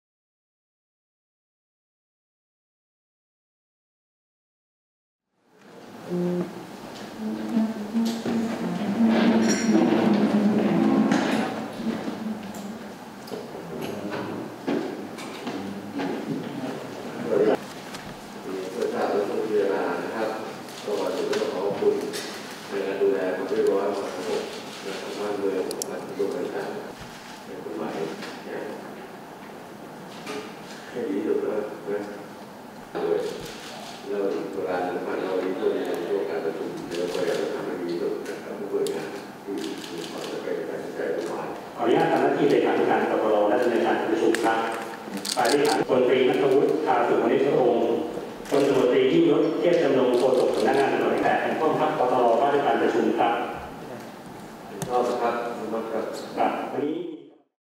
นายกรัฐมนตรี เป็นประธานการประชุมคณะกรรมการข้าราชการตำรวจ (ก.ตร.) ครั้งที่ 5/2565
นายกรัฐมนตรี เป็นประธานการประชุมคณะกรรมการข้าราชการตำรวจ (ก.ตร.) ครั้งที่ 5/2565 ณ ตึกภักดีบดินทร์ ทำเนียบรัฐบาล วันที่ 30 พฤษภาคม 2565